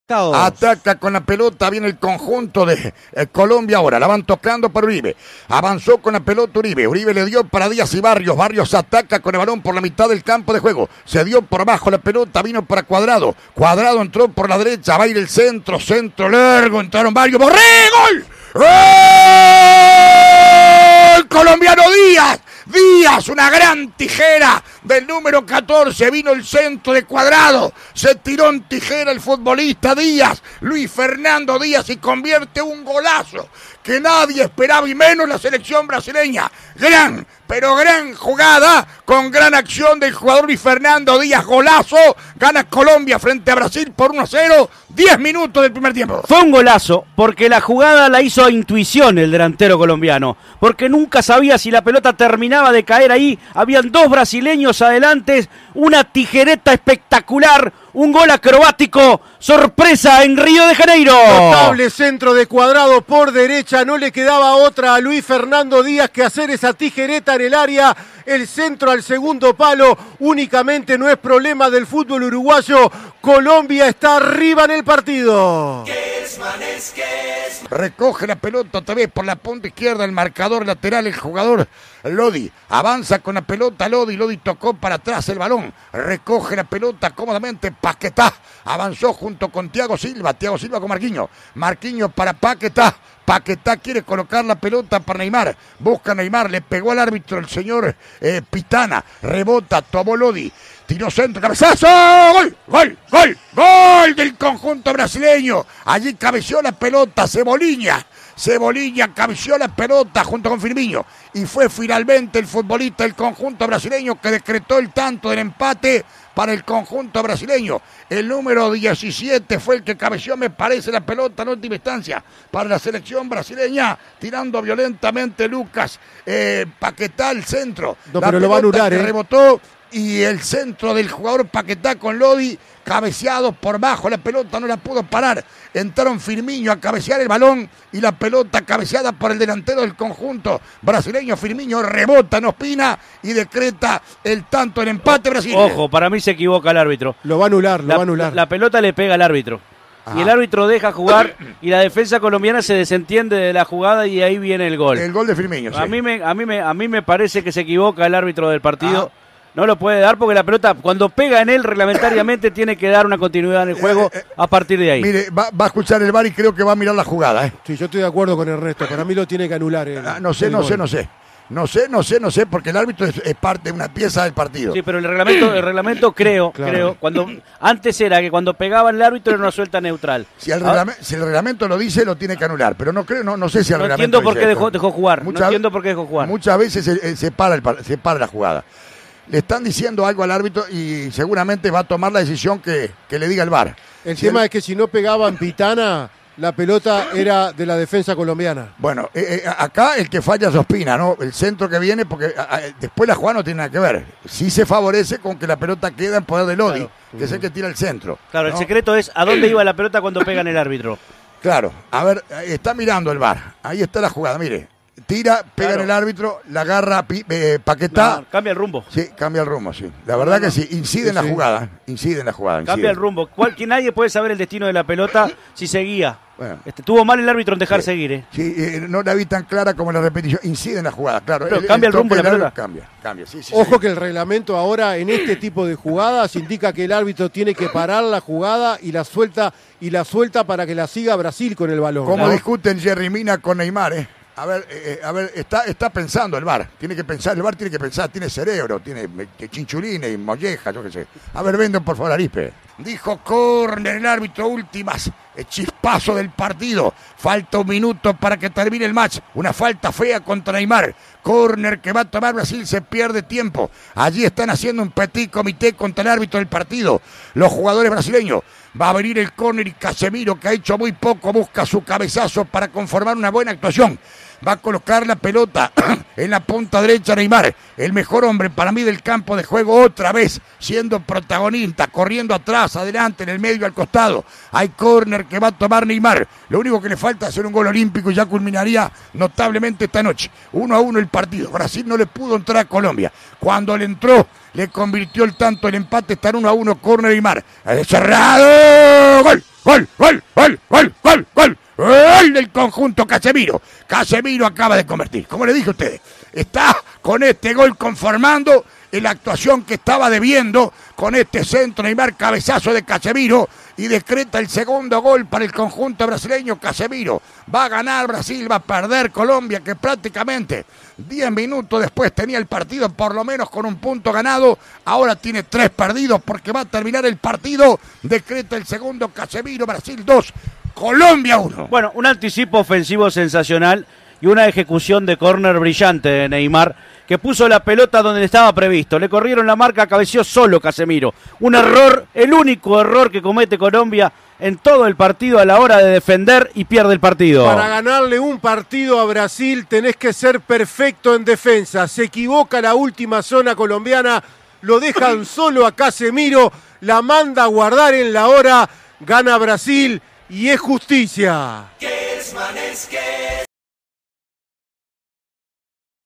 Brasil 2 Colombia 1 - Copa América 2021 (por Alberto Kesman)
Los goles del partido, por Alberto Kesman junto a Ernesto Faría y Richard Arce para CX 22 Radio Universal 970 AM.